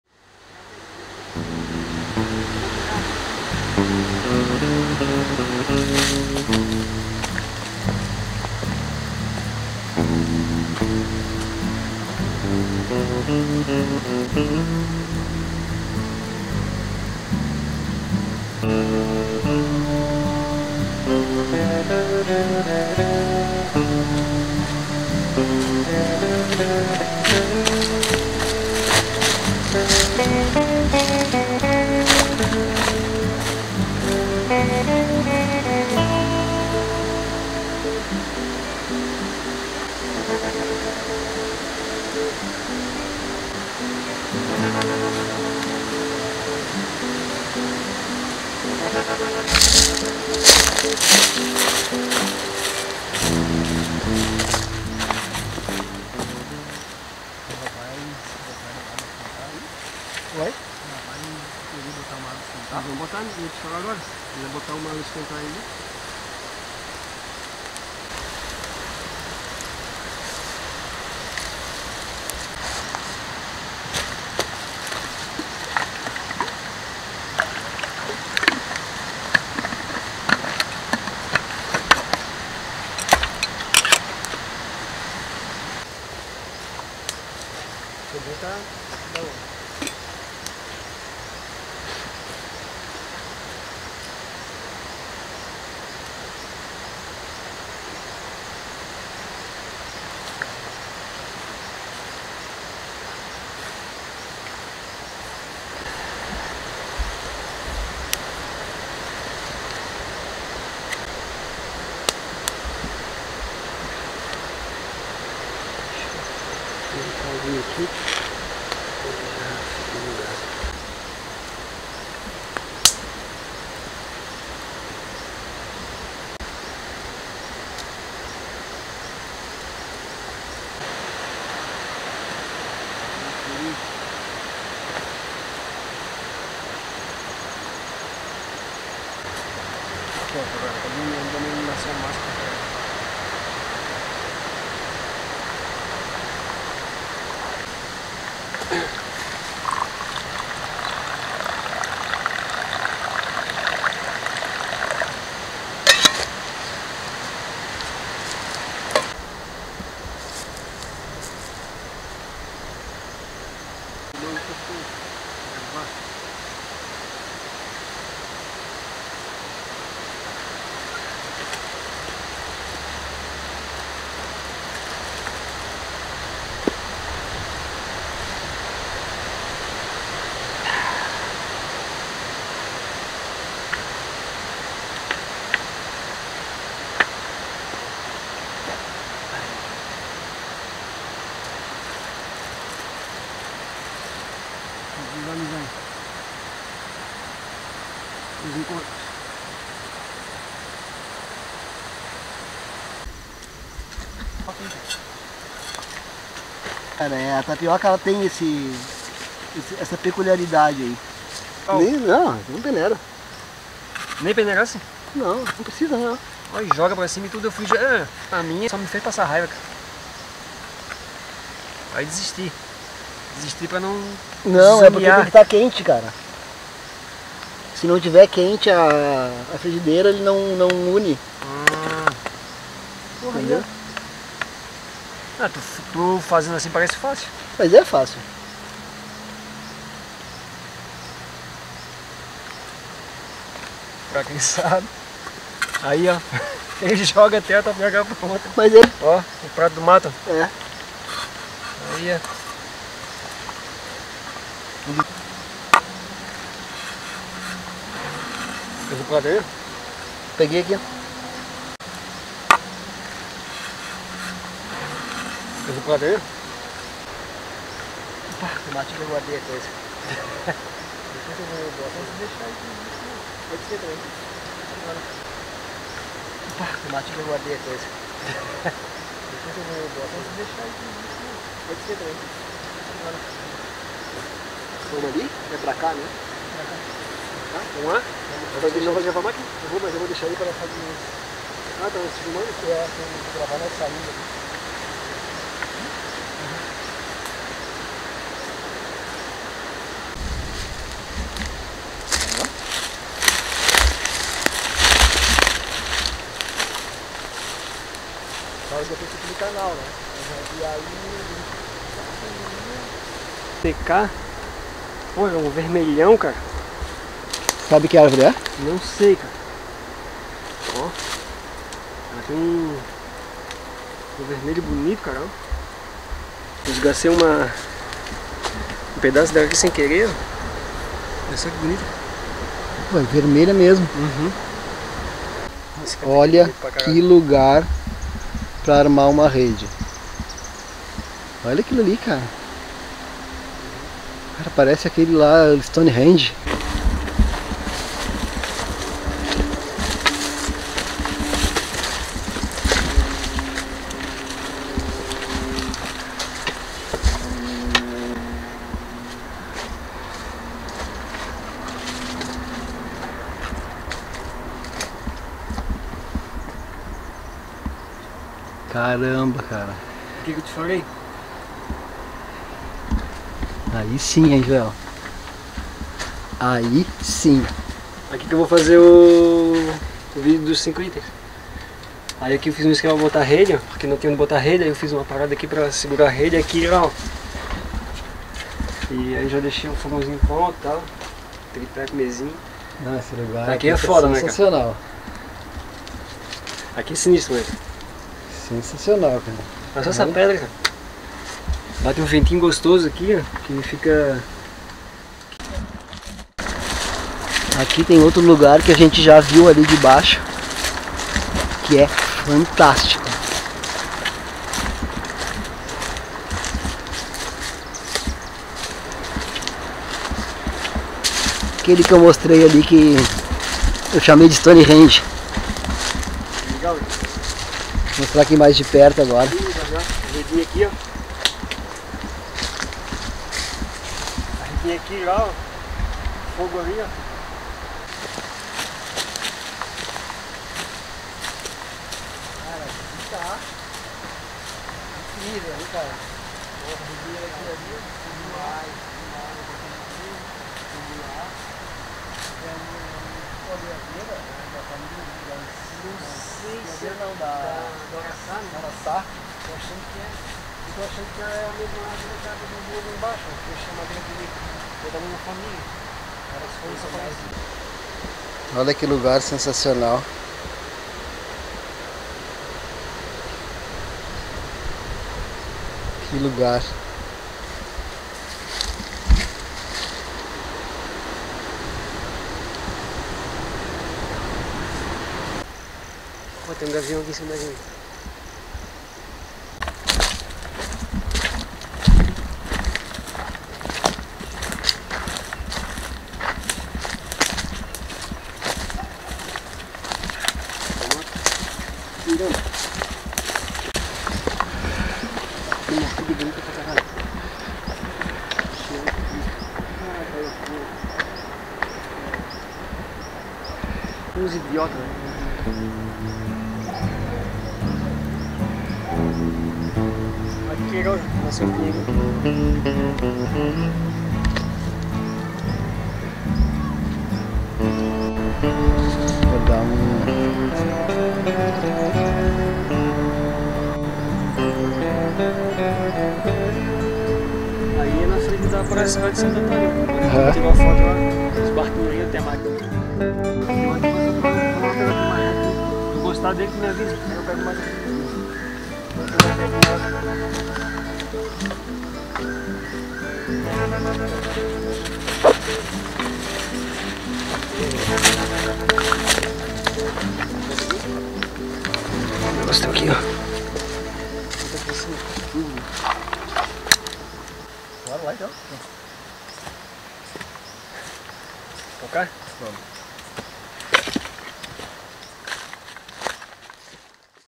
LUGAR INCRÍVEL PRA ACAMPAR / A CAVERNA DO PEABIRU
DOAÇÕES COM PIX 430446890/15 cpf banco inter

Fui passar uma tarde na  área de acampamento de um grande amigo, joel cordeiro do canal peabiru arte mateira, e fiquei encantado com o lugar, área de morro da mata atlântica, com uma vista incrível do ocano e de algumas praias, dependendo do mirante, que são vários.
uma pena que foi apenas uma trade e não dá para fazer quase nada. ainda mais na companhia desse grande amigo.  essa é uma das coisas que mais preso hoje em dia, estar na companhia de pessoas tão agradáveis e amigas, pois hj em dia sou uma pessoa que presa muito a amizade.

PRESERVE A NATUREZA
TRATE BEM OS ANIMAIS
AGRADEÇO POR ASSISTIR
A GENTE  SE VÊ NO MATO